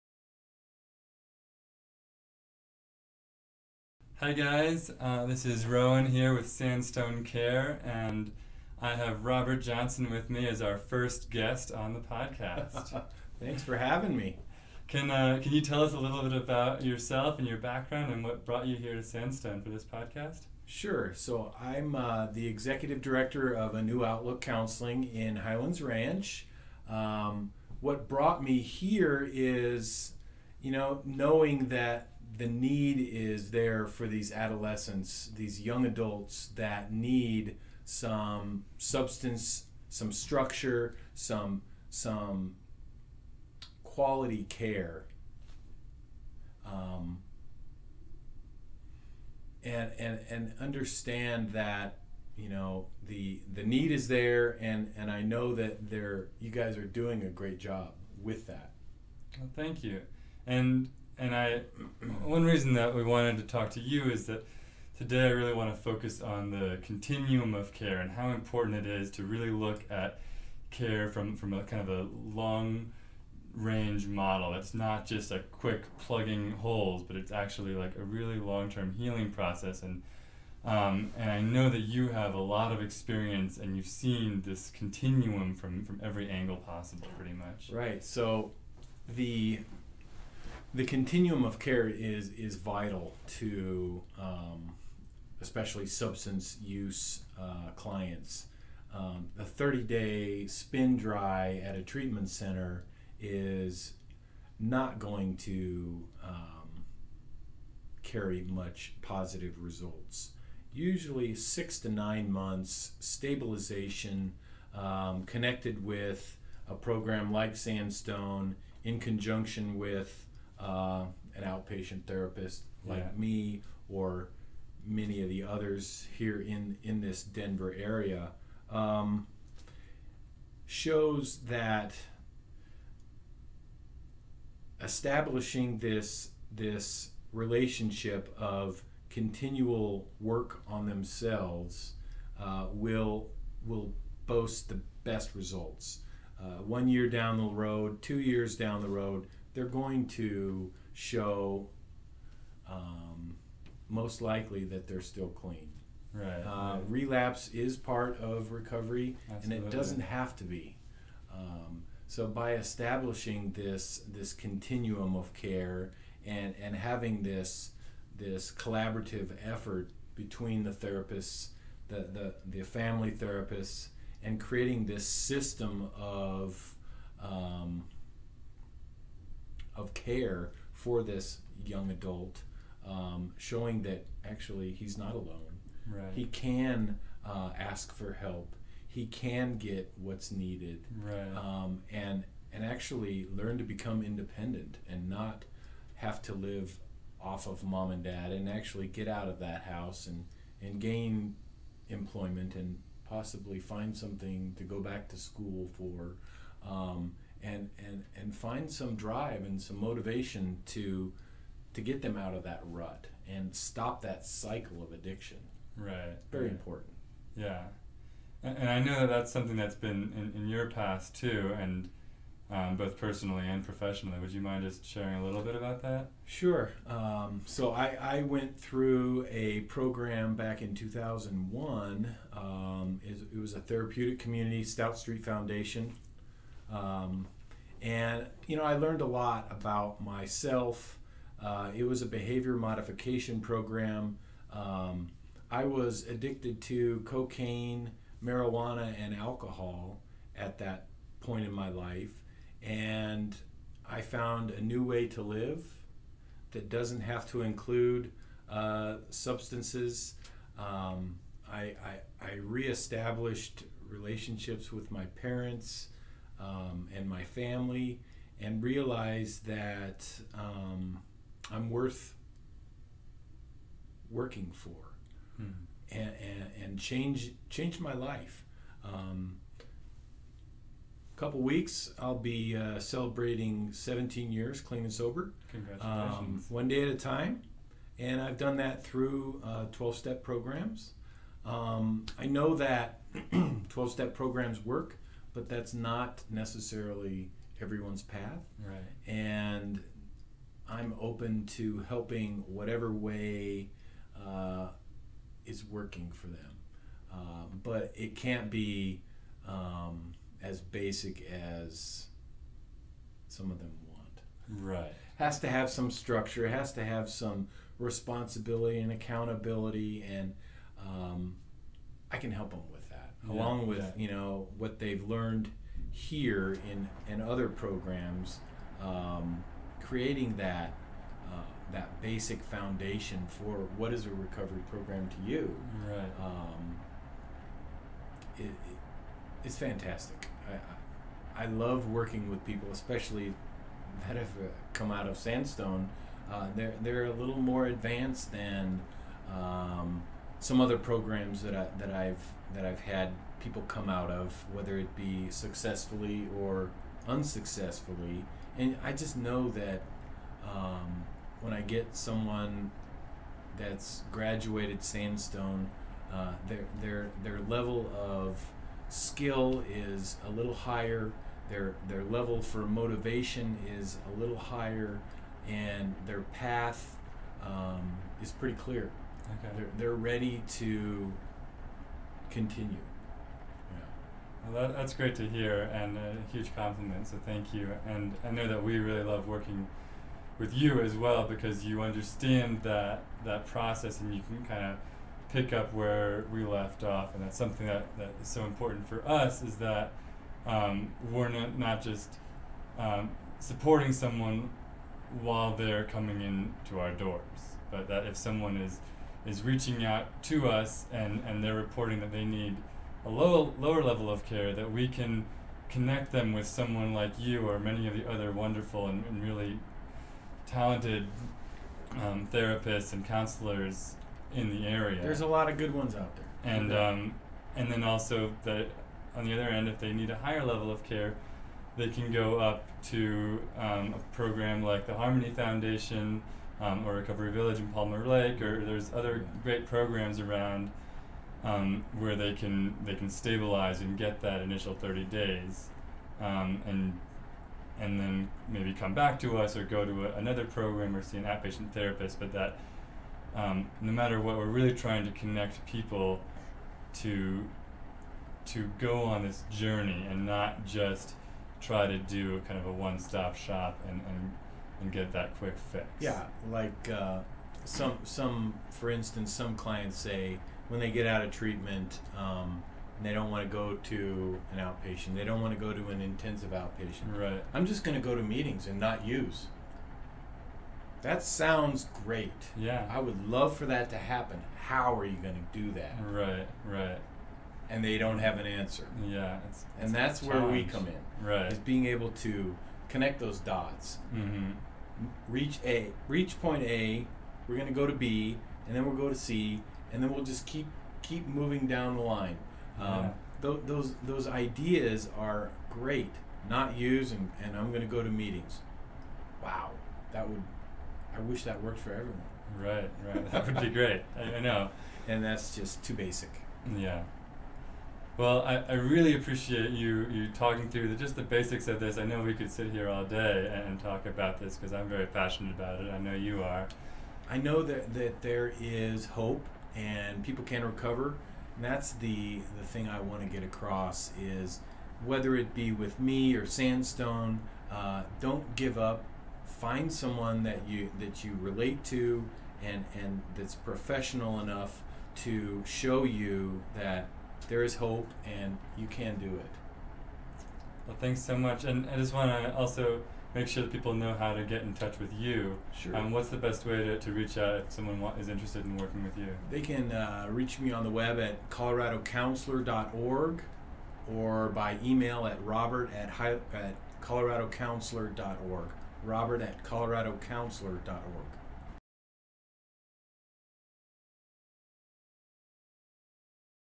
Robert Johnson Interview
Robert Johnson is a veteran of the recovery community and understands the importance of full-continuum treatment for long-term success in recovery. We define the continuum of care and Robert goes over how he collaborates with other providers to make sure that people are getting the best possible support.

Looking for help?